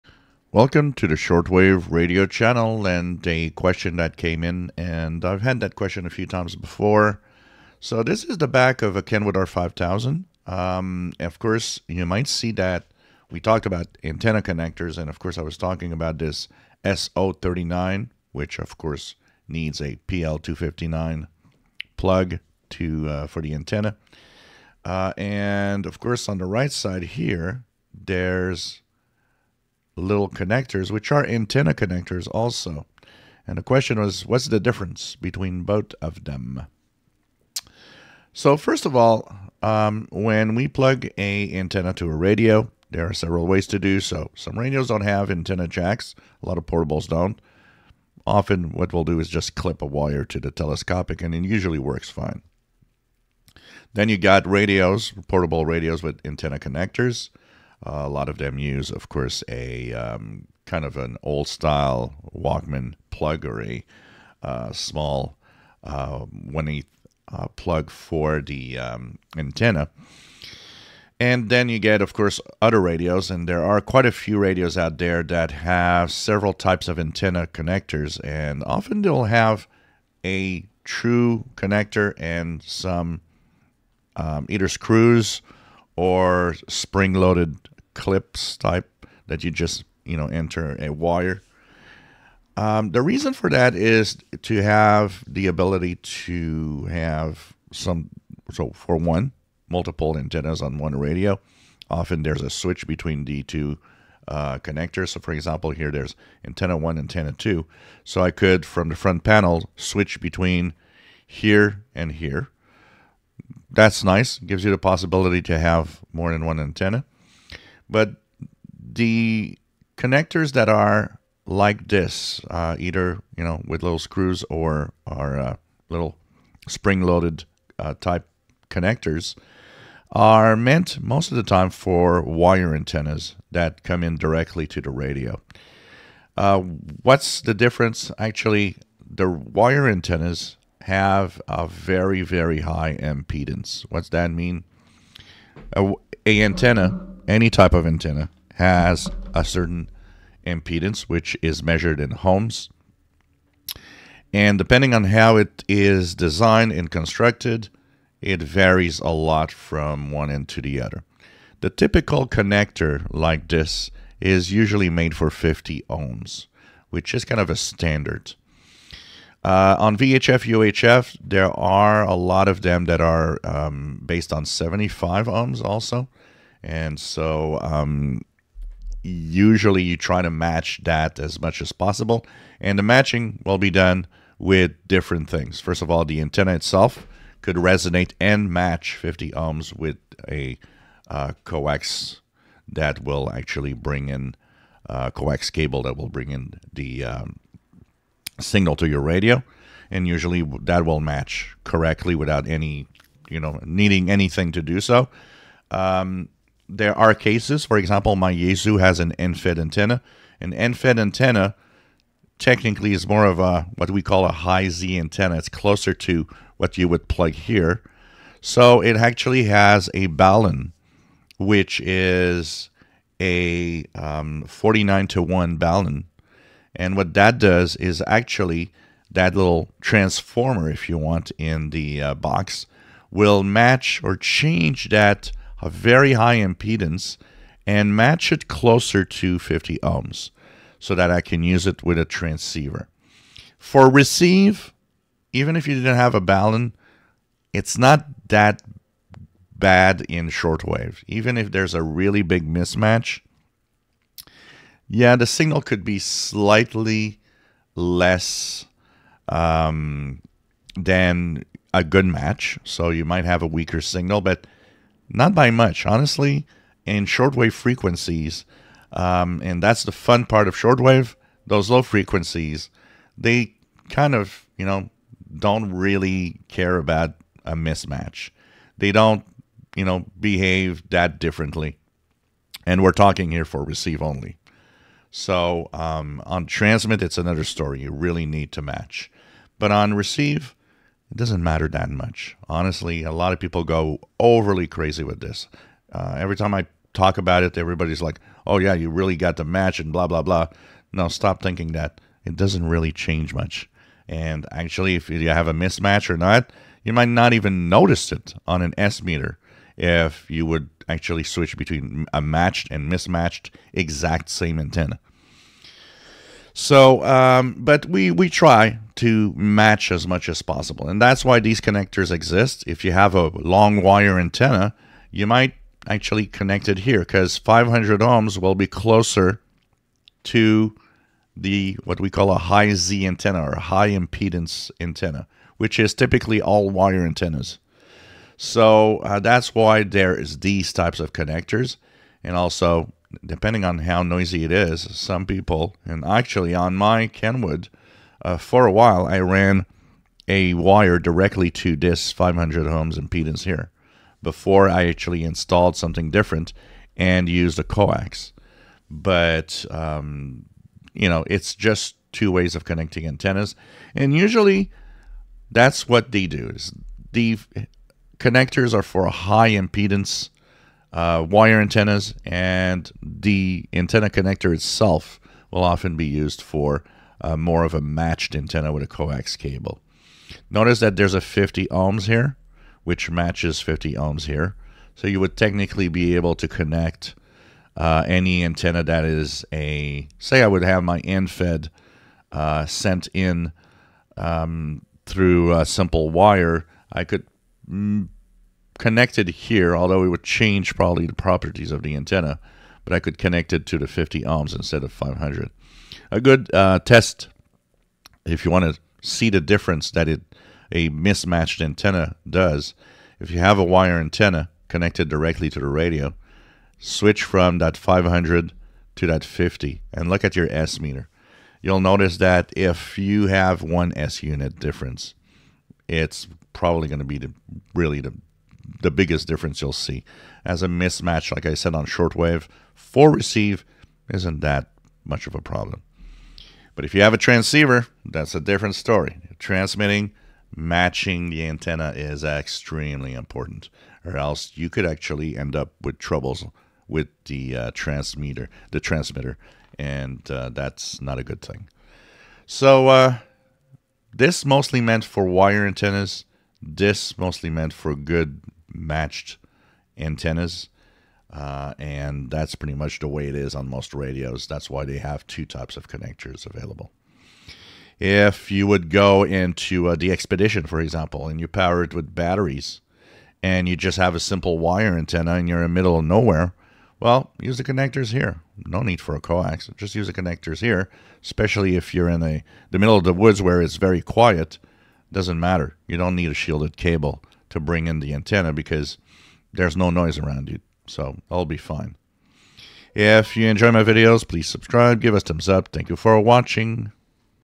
When a radio has more than one antenna connector like Kenwood R 5000
Many radios will have more than one antenna connector, a dedicated plug and often screws or clamps that accept wire antennas